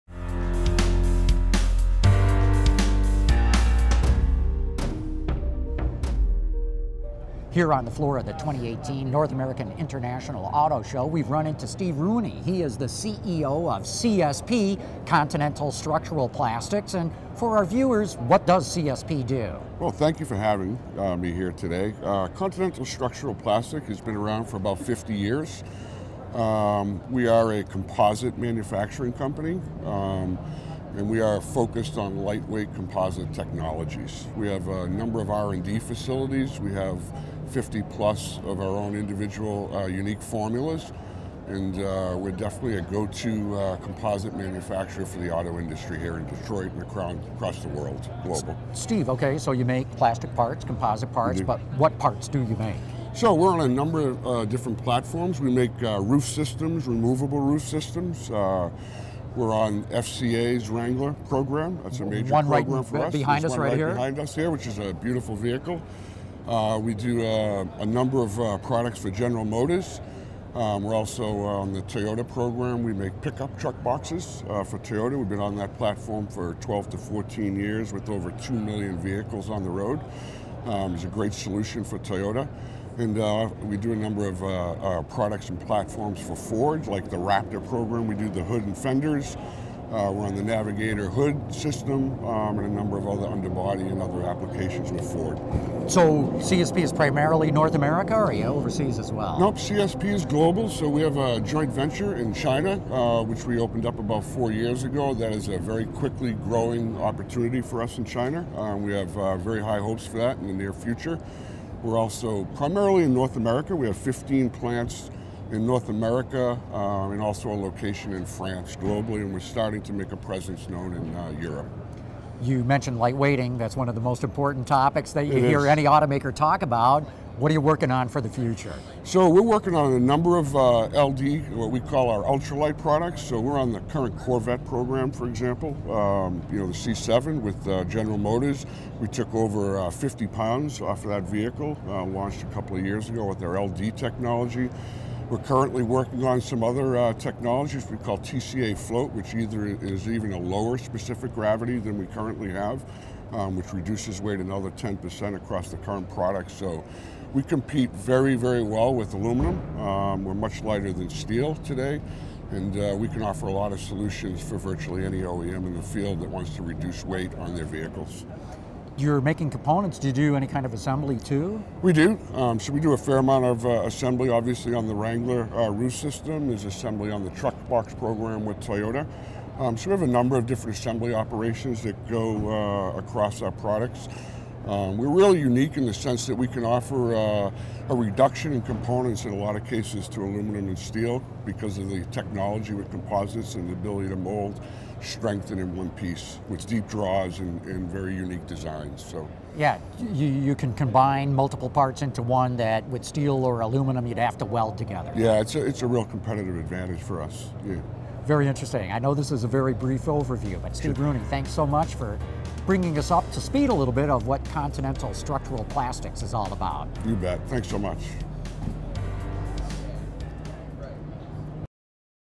CSP: 50 Years of Lightweighting - NAIAS 2018
Continental Structural Plastics is a composite manufacturing company that’s been around for 50 years, making parts for Jeep, Toyota and Ford just to name a few. Steve Rooney is the CEO of the company and he says their composites are even lighter than steel.